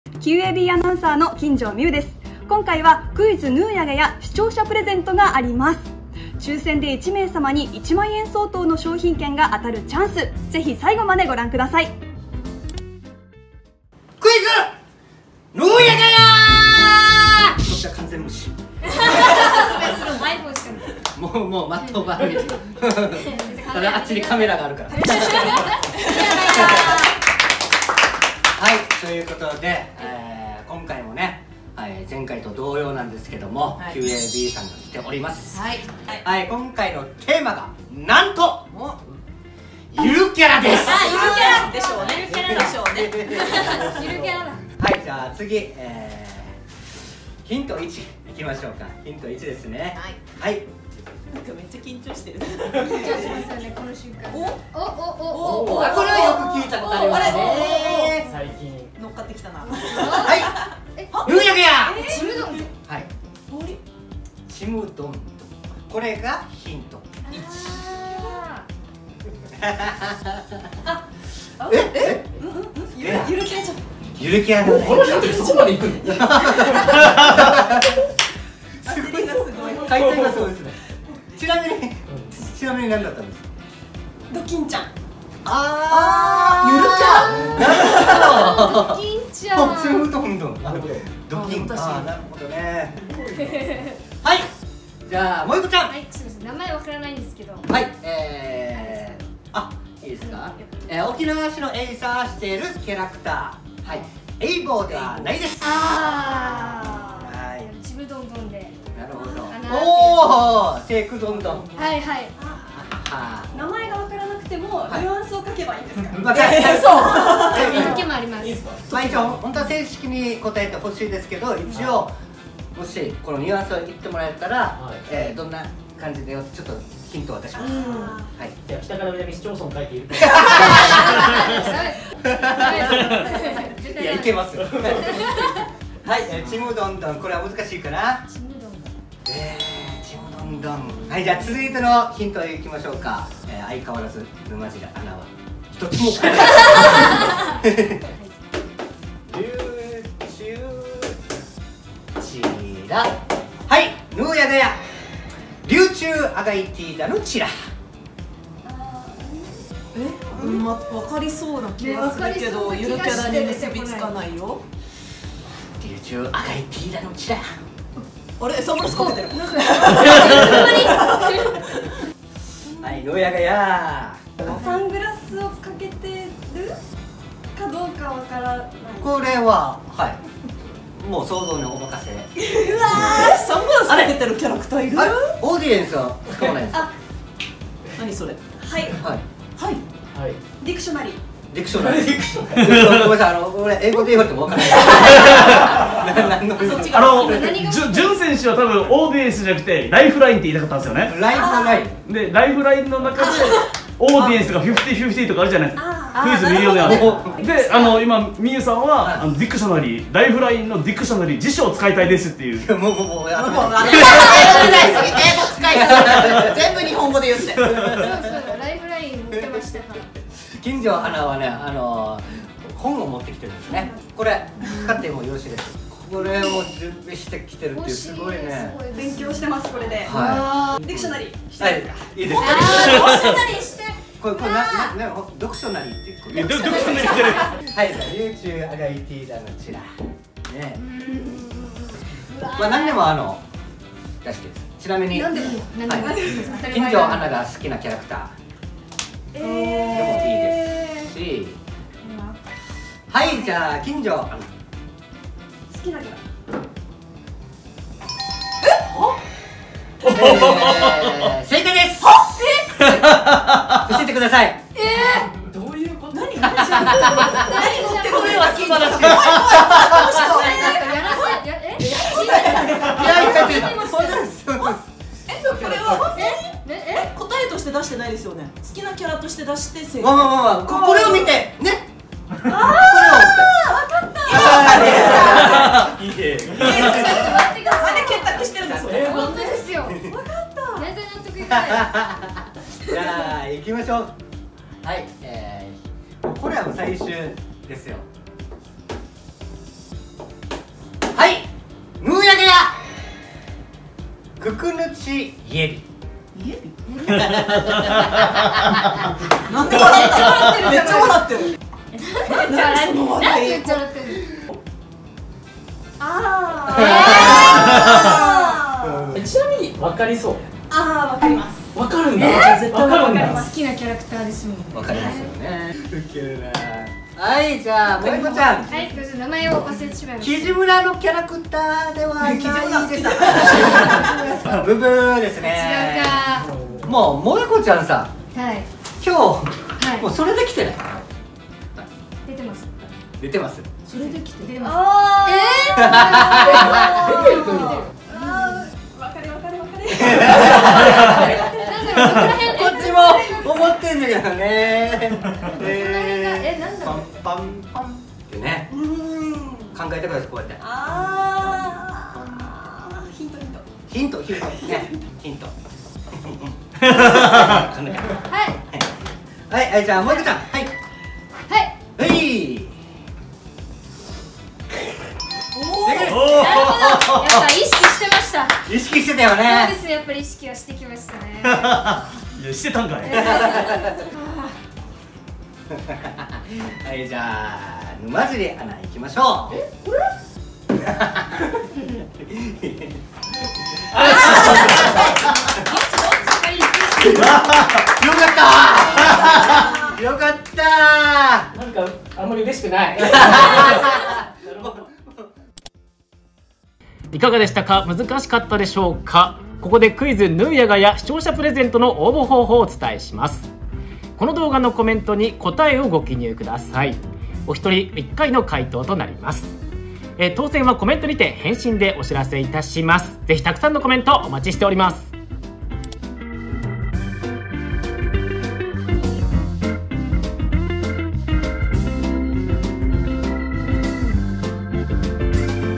ちむどんどんって実はゆるキャラだった？【視聴者プレゼント】
★応募方法★
答えが分かった方は、こちらのYoutube動画のコメント欄に答えをご記入ください！
回答はお一人様一回のみとなっております。
当選された方には、コメントいただいたコメントへ返信予定ですので、チェックをお忘れなく✨
締め切り：5月15日（日）までのコメント
当選発表：当月中
はいさい🌺
本日は第二弾！視聴者さん参加型クイズ！！
みなさんのご参加、どしどしお待ちしております♪♪

今回は前回に引き続き、QAB琉球朝日放送さんのコラボ企画「ぬーやがやー」👏👏
回答者には「しまくとぅば検定」を持っている「金城美優アナウンサー」と「沼尻和樹アナウンサー」！

うちなー芸人のじゅん選手が、うちなーぐち（沖縄方言）を使ってクイズを出題するシリーズ「ぬーやがやー」！
今回のテーマは「ゆるキャラ」！！
じゅん選手出題の3つのヒントで、どのゆるキャラを説明しているかを考える、沖縄方言クイズです♪
みなさんも一緒に考えながらお楽しみください^^

おきなわちゃんねるの回答者には、沖縄出身の女優「ひーとー」、沖縄のタレント「もえこ」の2人が、じゅん選手のクイズに挑みます！！

*沖縄の方言「ぬーやがやー」は、「何でしょうか」の意味ですよ♪
**「しまくとぅば検定」とは、沖縄の言葉を未来へ繋いでいこうと実施されている検定のこと

これからも沖縄の文化や方言、観光の魅力などを伝えていきます！

【おきなわちゃんねるSNS 】